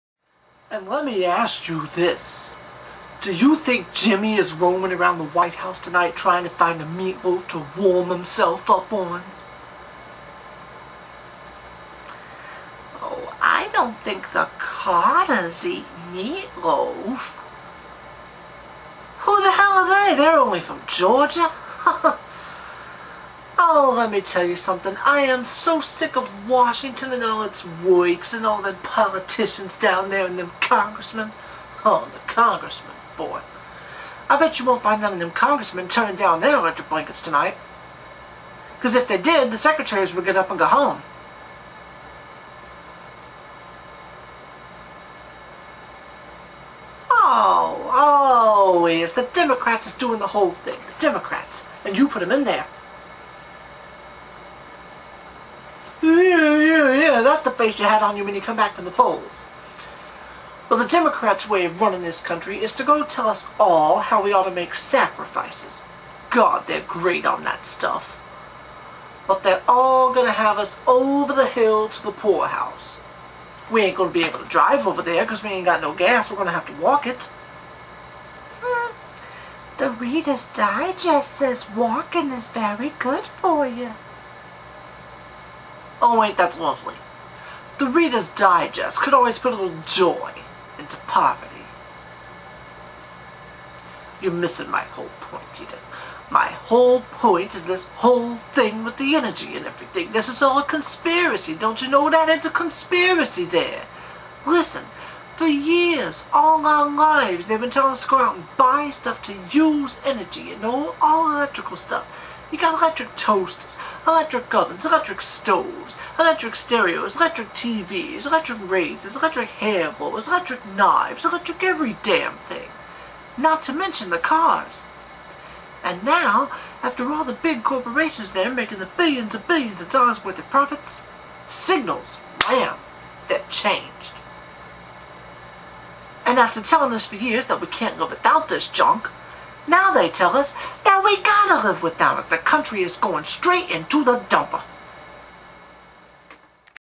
My Reenactment of Archie Bunker on Democrats
I memorized the whole speech.